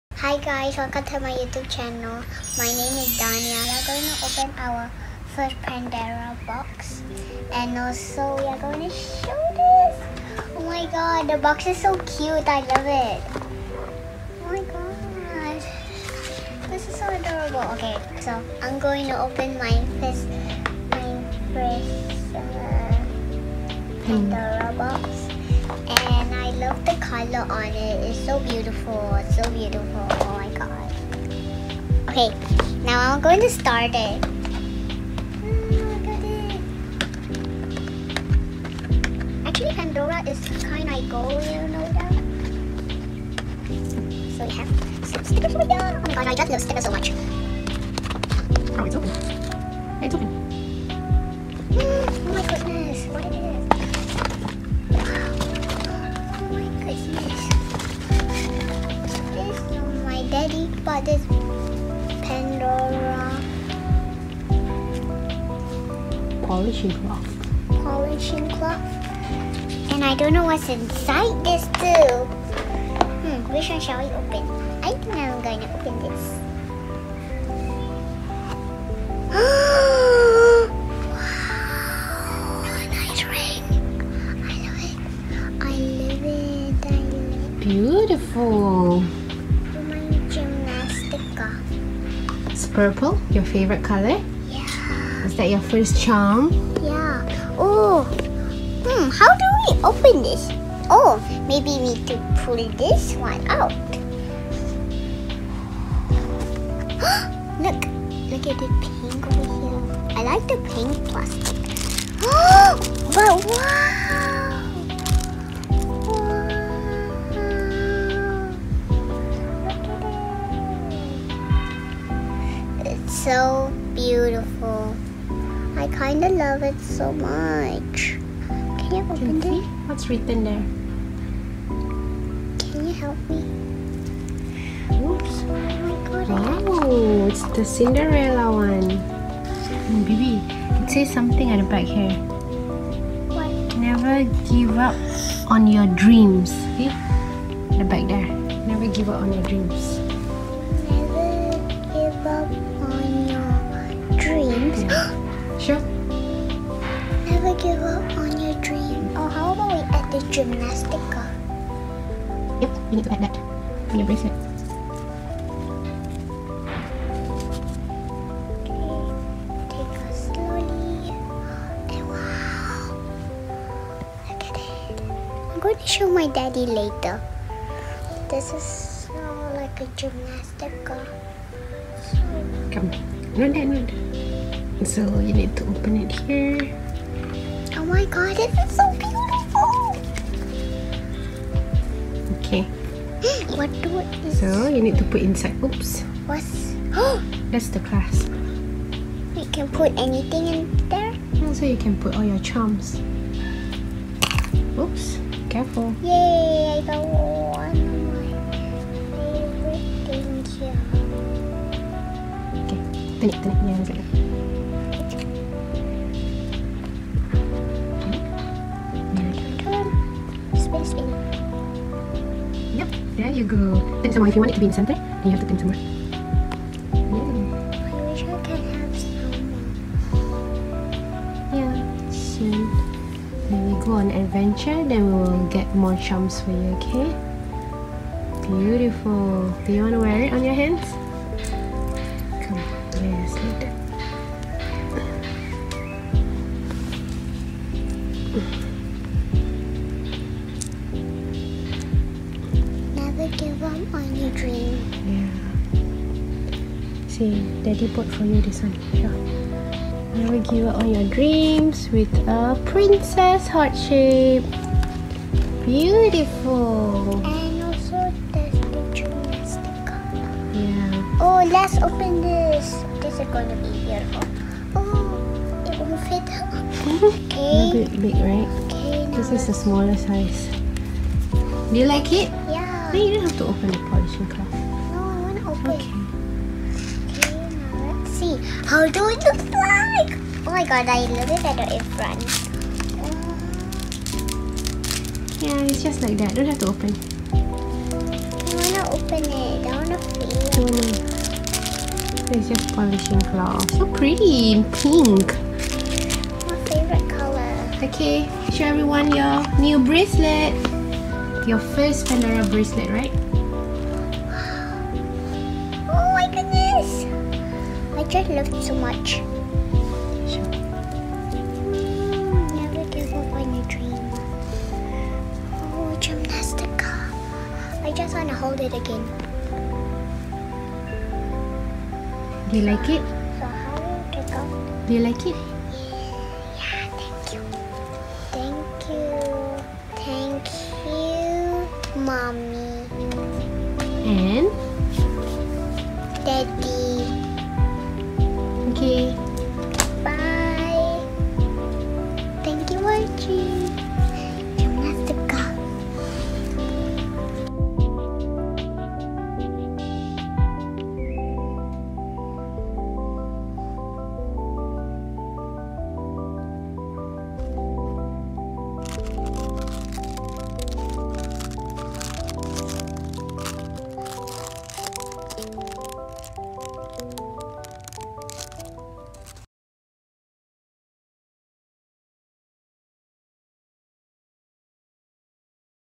UNBOXING MY FIRST EVER PANDORA! | princess bracelet & new charms
Hello everyone!
My parents got for me my first ever pandora bracelet and charms and I'm really loving it so much! It's still a little big but mommy got me the most beautiful bracelet and charms to remind me to never give up my dreams. Hope you enjoy watching my video.

Love, Daania